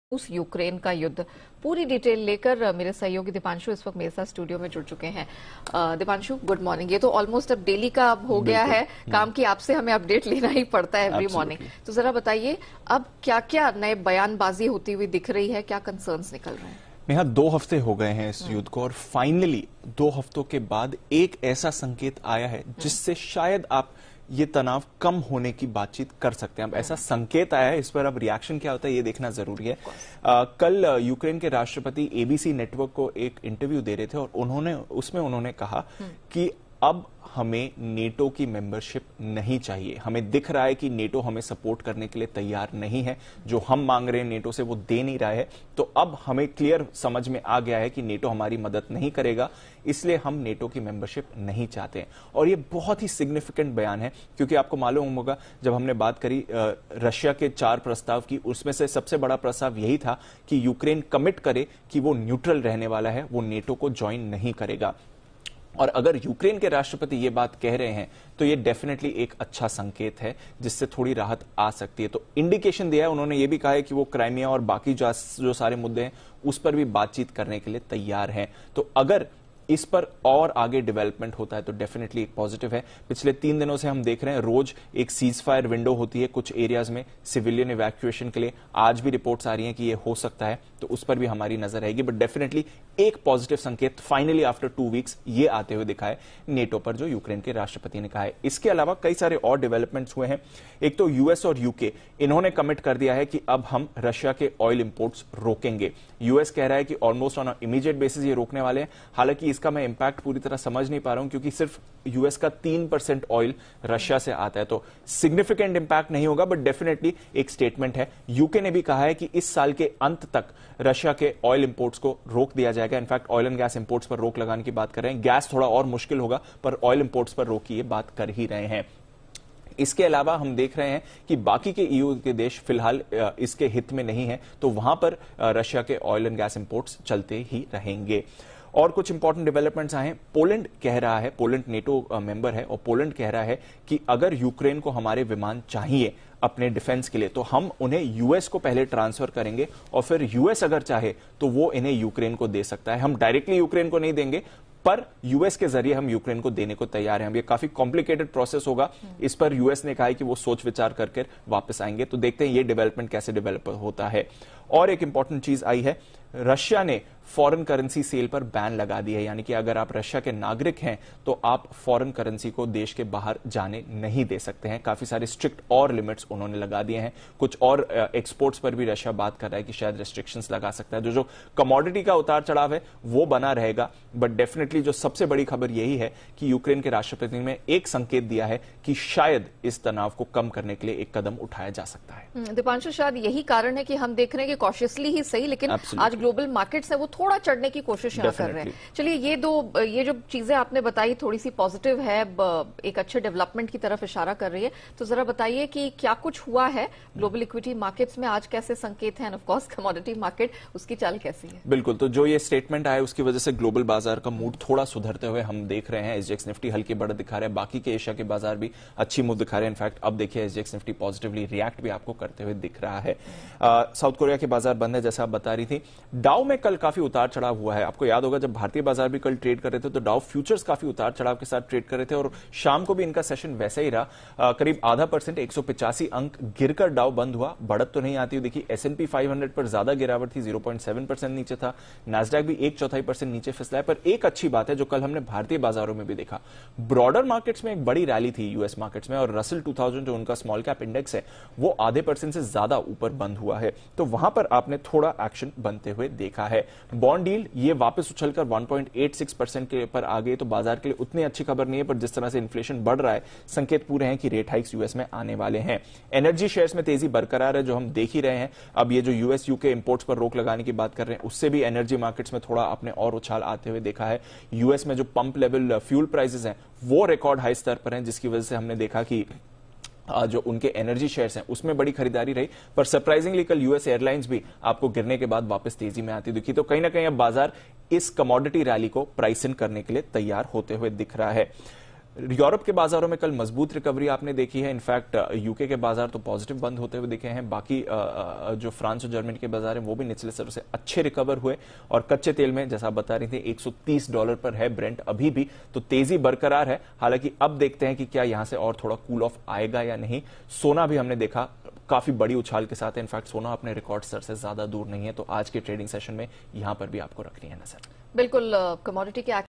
Dow Jones Falls 185 Points; Apple's New 5G iphone SE2022 launched but stock 1.5% down; gold at $2050
🔸💹GlobalMarket से बेहतर संकेत
SouthKorea बाजार आज बंद
DowJones 185 अंक फिसलकर बंद
Russell2000 0.6% उछला
📱Apple का नया iPhoneSE2022 लॉन्च,पर शेयर 1.5% नीचे
Gold $2050 के ऊपर